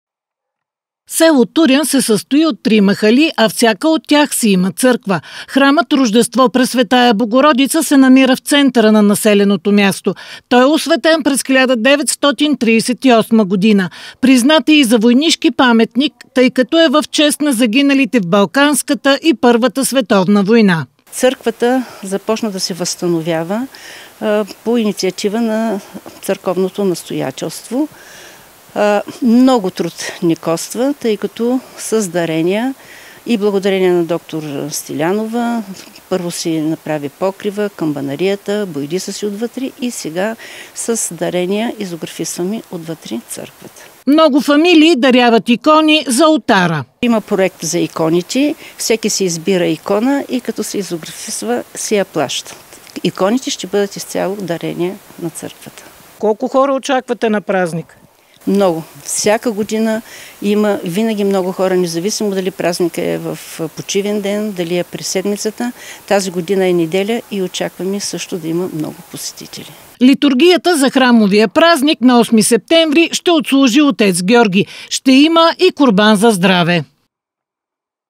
Храмов празник ще отбележи църквата "Рождество Богородично" в село Турян на 8 септември
Храмът в смолянското село Турян е напълно обновен с дарения от местните хора. На 8 септември жителите на населеното място се готвят да отбележат и празника на църквата си „Рождество Богородично“.